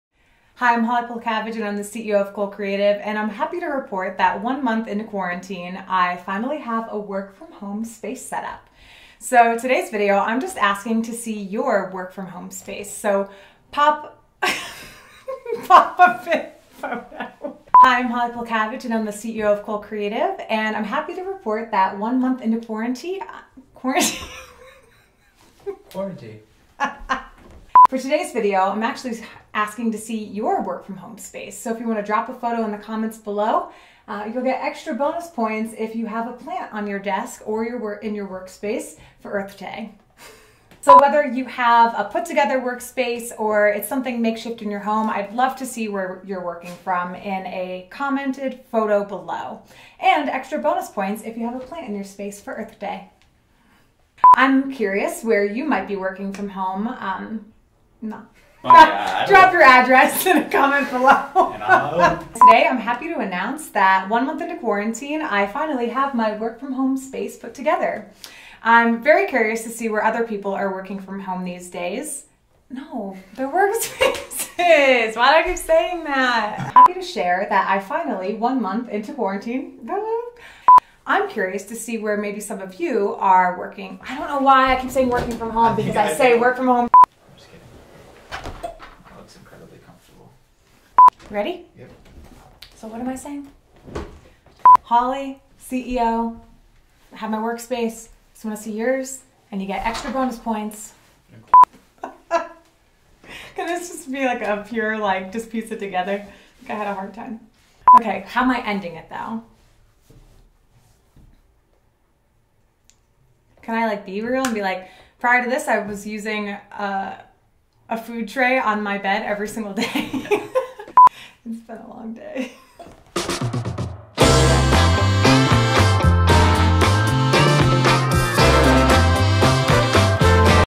Staying Connected | Day Twenty Two | Coal Creative
We're about a month into quarantine and Holly is FEELING it. Here's a not-so-typical daily video about her new workspace but mostly a behind-the-scenes look at how all of these videos actually go. Enjoy!

*The real ask of this video is to show us your workspace - no matter how makeshift! Bonus points for plants for Earth Day!*